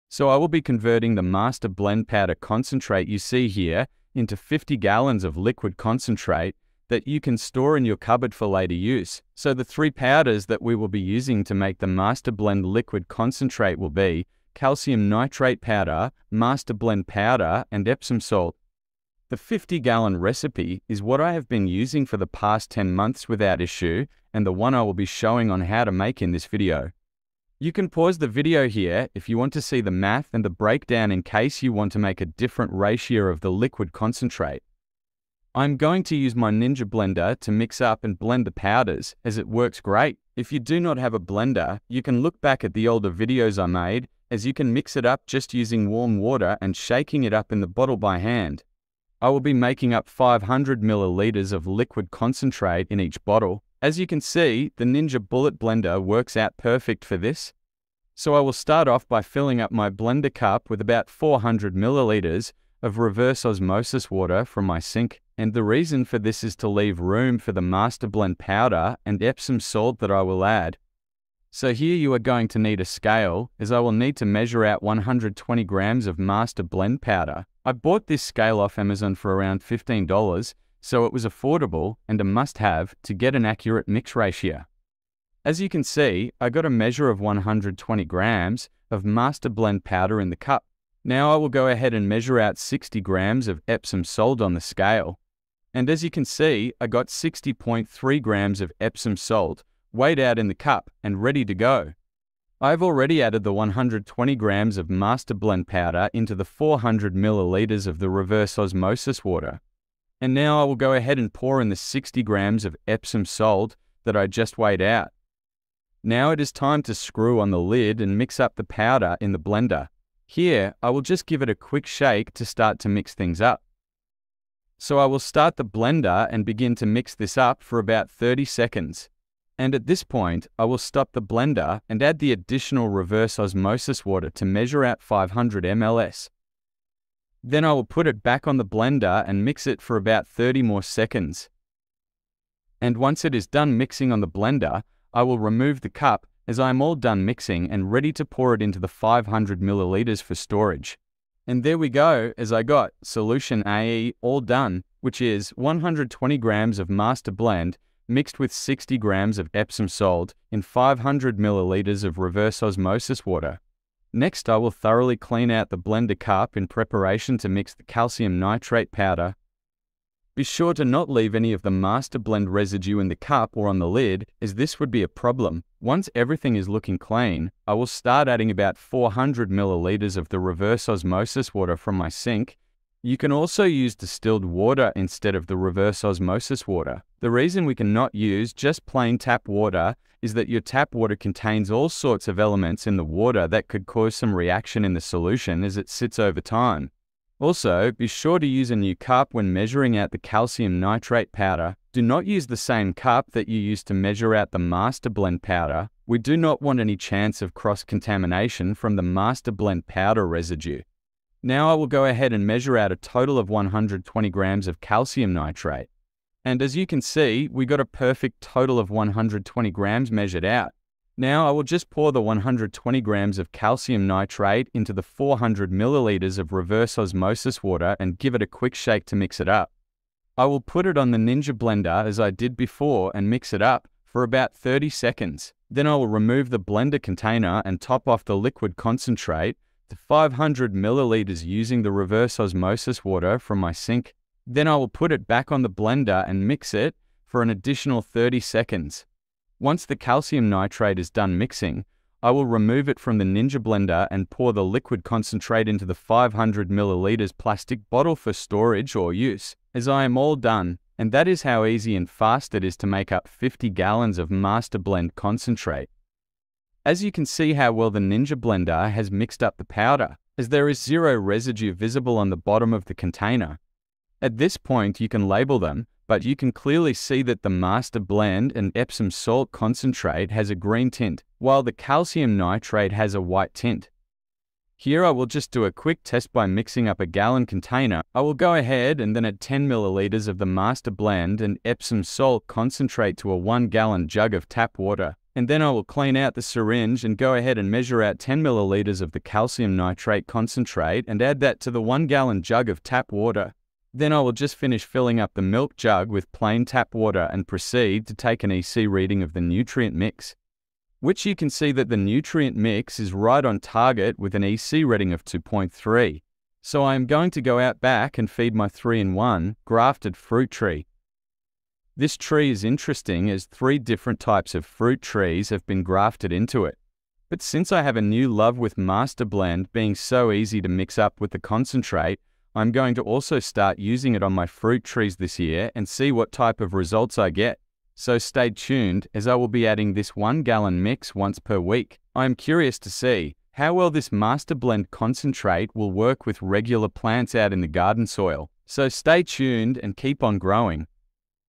How to Convert MasterBlend Powder into 50 Gallons of Liquid Concentrate - Part 3
This tutorial will teach you on How to Convert your MasterBlend powder into a premixed Liquid Concentrate for 50 gallons for later use. This saves so much time and makes using MasterBlend extremely easy while still benefiting from the savings of the original powdered product. Once you do a batch you will never go back to the original way you were mixing your MasterBlend. Just make sure to keep the product away from light when storing and it should be good for at least 1 year.

30ml Syringe with Blunt Needle Tip 1.5 Inches

Scale - The one in video that I Use

WHAT IS COST OF MASTE BLEND PER GALLON?

$59.99

10 lbs of MasterBlend1
10 lbs of Calcium Nitrate
5 lbs of Epson Salt

453 grams in 1 pound

2.4 grams of MB are needed per gallon
2.4 grams of Calcium Nitrate are needed per gallon
1.2 grams of Epsom Salt per gallon

453 divided by 2.4 = Would make 188.75 gallons per pound
188.75 gallons x 10 pounds = 1887.50 total gallons
$60.00 divided by 1887.50 = 0.0317

So if my math is right it would cost a little over .03 to make a gallon of full strength master blend..

The shipping is free and there is no tax as it comes with all 3 things that are needed..

Does that seem right??? Pretty cheap in my opinion!!!

Master blend = Maximum Solubility: 3.5 lbs. Per gallon.
Cal Nitrate = 1020g per L
Mag Sulfate = 850g per L